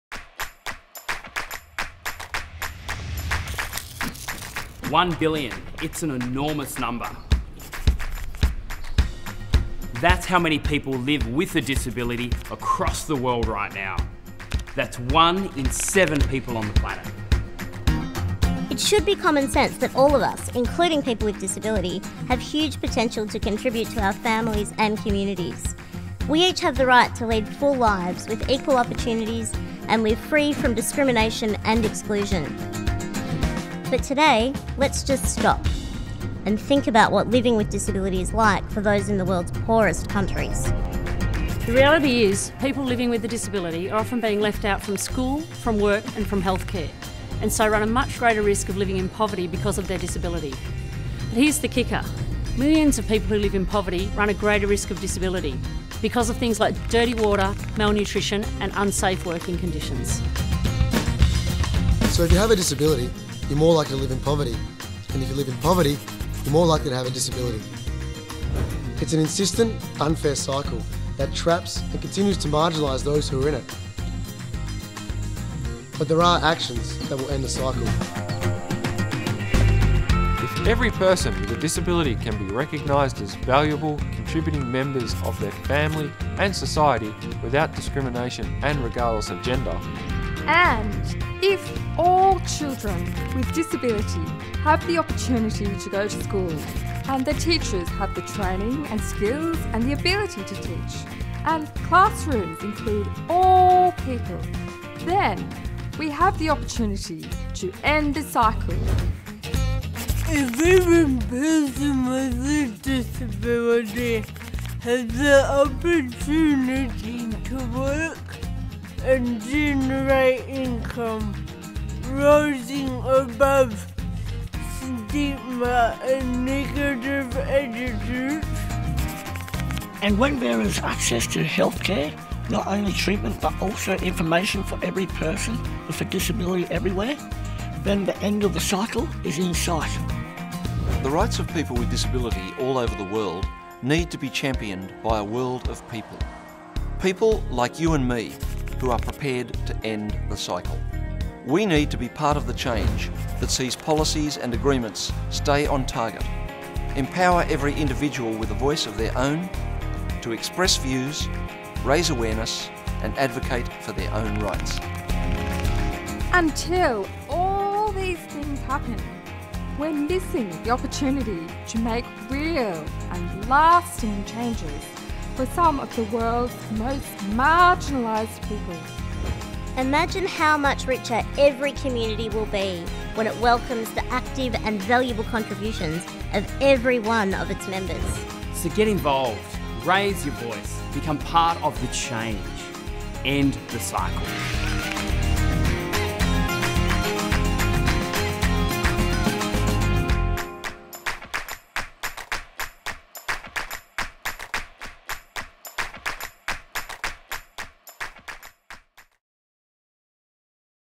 End The Cycle of Poverty and Disability - Explained
Poverty and disability go hand in hand creating a cycle that's hard to escape. Extreme poverty causes disability through factors like lack of access to good nutrition, adequate health care, clean water, or through unsafe working conditions. Disability can contribute to poverty through discrimination and attitudinal barriers. A person with disability is less likely to have access to rehabilitation, education and employment -- which could help end the cycle of poverty and disability.

Presented by Sam Cawthorn, Stella Young, Paralympians Liesl Tesch, Dylan Alcott and Grant "Scooter" Patterson, Australia's Disability Discrimination Commissioner Graeme Innes, Dale Sheppard, Elena Down and Stevie Wills.

Produced by Room3 - Film and Animation for not-for-profit organisations